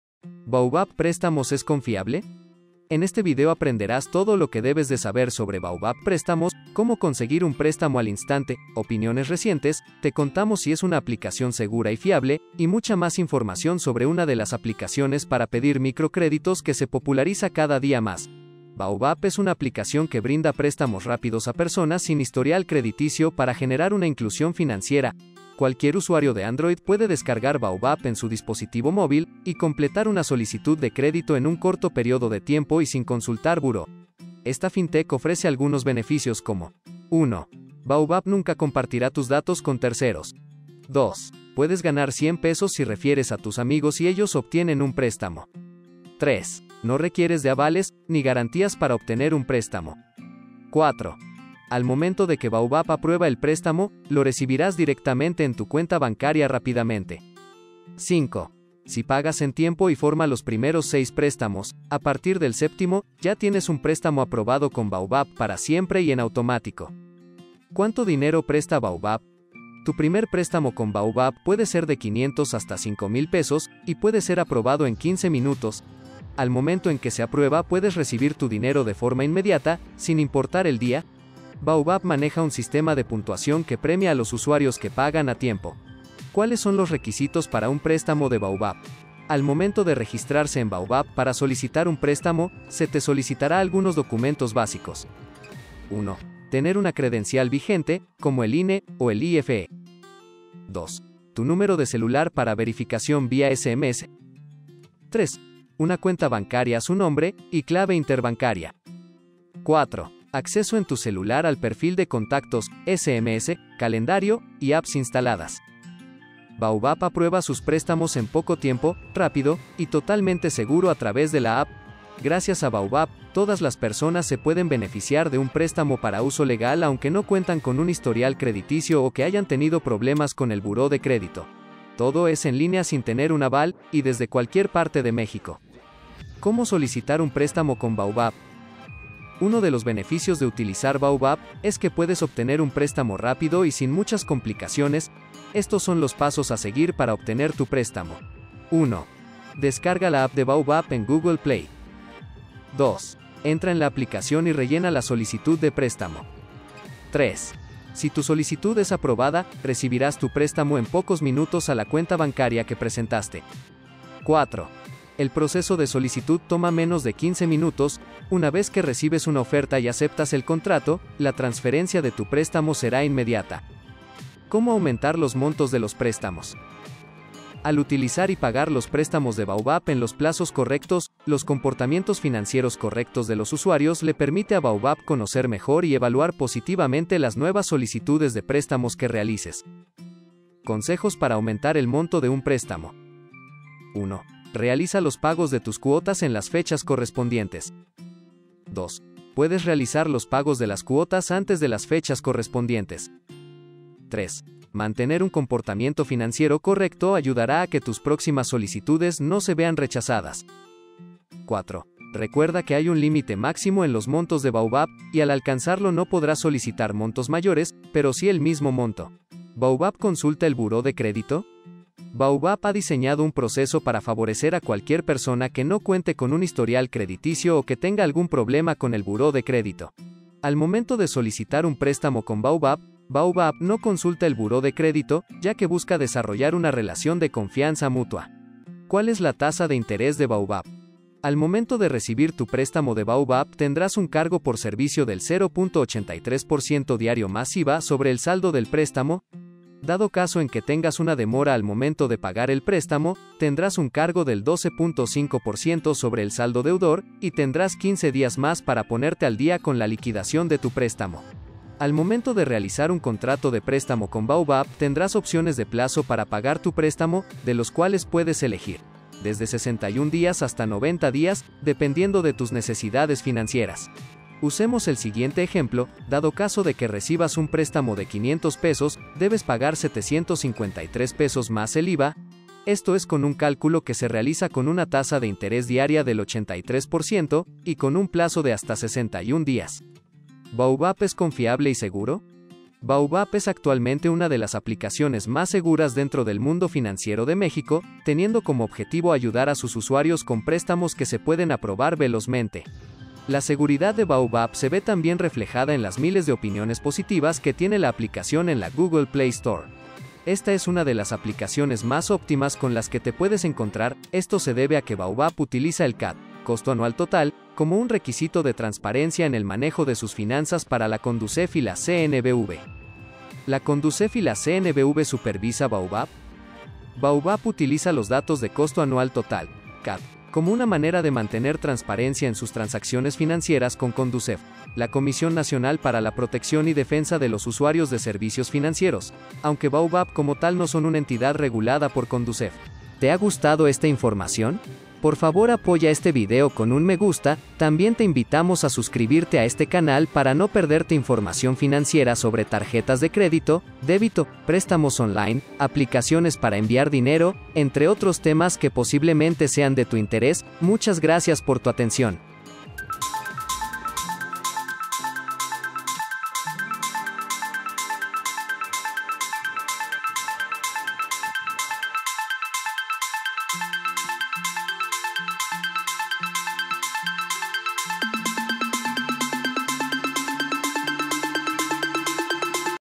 💰 BAUBAP PRÉSTAMOS ¿Es CONFIABLE la APP? 📲 OPINIONES | REQUISITOS ¿SIN BURÓ de CRÉDITO?
🔸 Te REVELAMOS si la APP de Préstamos BAUBAP es Confiable o No. Descubre las opiniones sobre la Aplicación, sus requisitos, y más.

🔸 Dicen que Baubap no checa Buró de crédito para la aprobación de un Préstamo, ¿Esto es verdad? Te contamos en este video.

🔸 Además, compartiremos contigo los montos de dinero que puedes solicitar a Baubap, su tasa de interés, si es una entidad supervisada por la condusef, y todo lo que necesitas saber antes de solicitar tu préstamo.

¿Tienes algún comentario? Escribe abajo y te responderemos.

Si te hemos ayudado con el video, por favor, dale un me gusta. 👍